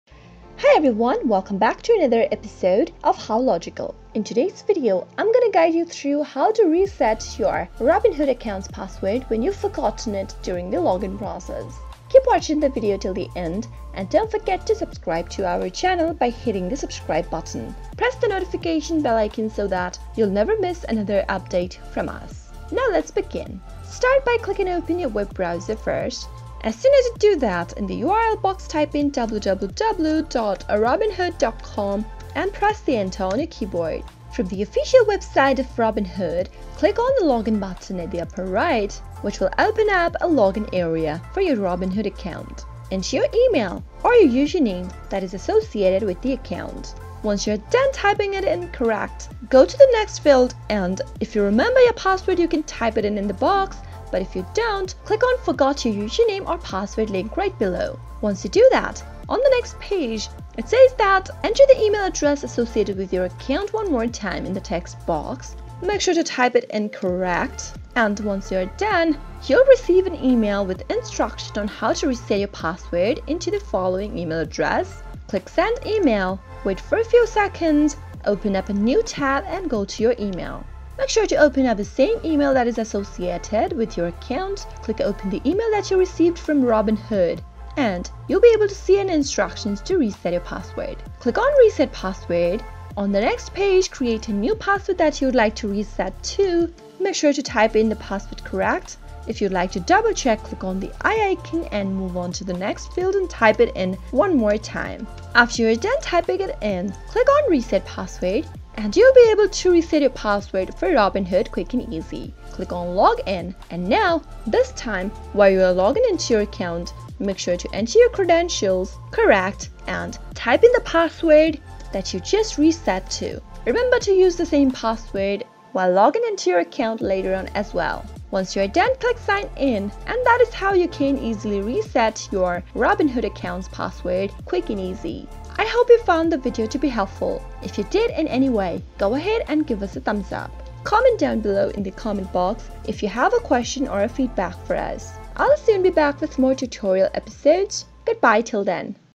How To Reset Robinhood Account Password? Change Robinhood Password 2021
Don't know how to reset your account's password for Robinhood? Do not worry this video will guide you on how you can reset your password. Robinhood is an American service company headquartered in Menlo Park. It is known for offering commission-free trade of stocks and exchange-traded funds via a mobile application. Resetting your password for your account is a pretty good option when you are not sure about the security of your account and when you are constantly worried about someone else knowing your password for your account. To reset your password, you need to follow the step given below:
1. Open a web browser.
2. Go to the URL box and type in and press Enter. You can also do this on your mobile phone. You will end up on the official website of Robinhood.
3. Click on the Log-in button at the upper right corner of the page.
5. Type in your password. If you remember your password, type it in the box. If you don't, click on Forgot your user name or password link right below.
6. Make sure to type it correctly. You will receive an email with instructions on how to reset your password.
7. Click on Send an email.
8. Open up a new tab, and go to your email. Make sure to open up the same email that is associated with your account.
9. Click open the email that you receive from Robinhood.
10. Click on Reset password. On the next page, create a new password that you would like to reset
11. Enter the password you would like to change. Make sure to type the password correctly. You can double-check your password by clicking on the eye icon at the right corner.
12. Confirm the password by entering the password one more time.
13. Click on reset password.
14. Click on Log-in. Make sure this time, when you log in to your account,
enter your credentials correctly. Type in the password that you just reset. That's how you can reset your Robinhood account password quickly and easily.

0:00 - Intro
0:22 - How To Reset Robinhood Account Password?
1:15 - Outro